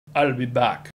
lipsync project
This is a lipsyncing project from college.  Completed in Maya 6.  Piedmont Community College, Yanceyville, NC, 2005.